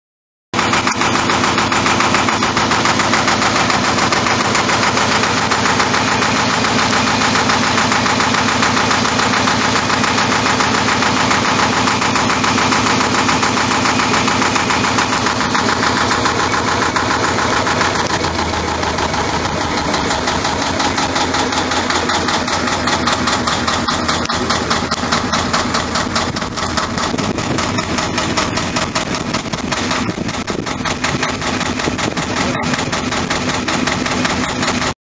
Lister HA2 Narrowboat Nutfield Crick 2018
Narrowboat Nutfield running it's Lister HA2 at Crick Boat Show 2018